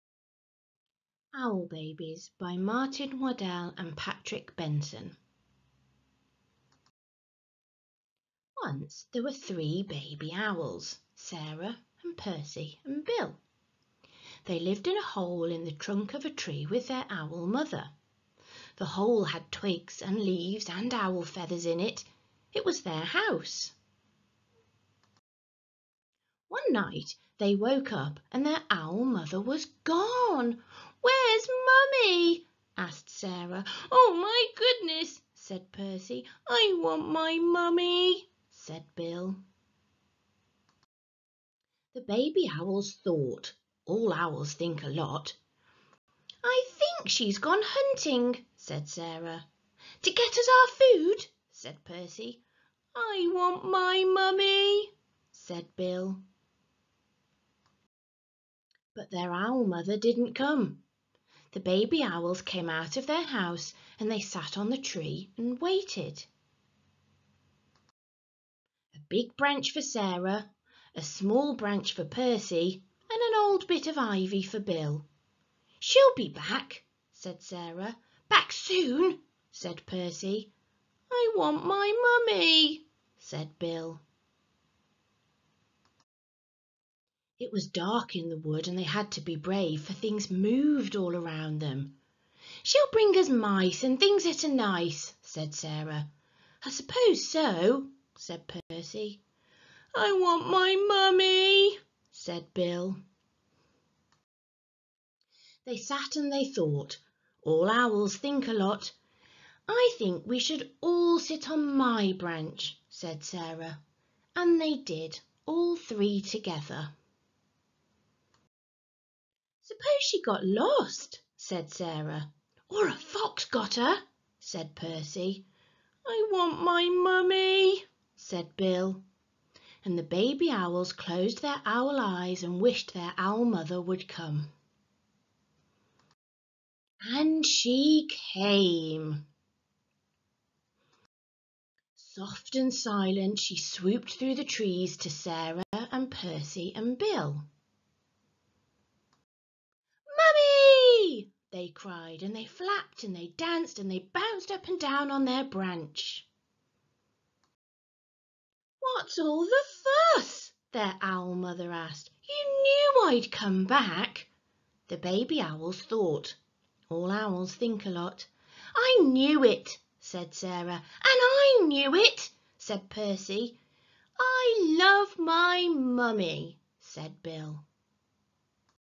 Owl Babies Read Along Story
A Read Along version of the popular story about the young owls and their mother, Owl Babies by Martin Waddell and Patrick Benson. Children can listen to the story being read whilst following along on the screen or mute me and the children can read the story aloud themselves.

If you like this, then please have a look at some of my teaching resources that accompany this story.